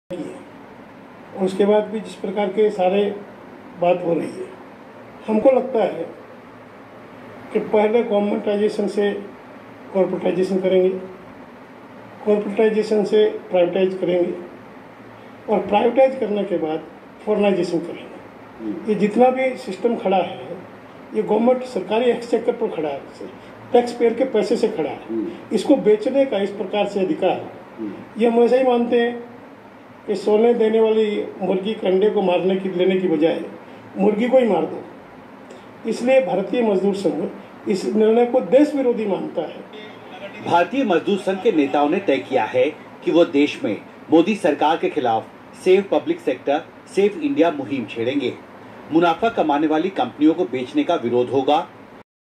Bharatiya Mazdoor Sangh (BMS)
Shri. Pawankumar Zonal org. Secy BMS interview in NDTV against Privatisation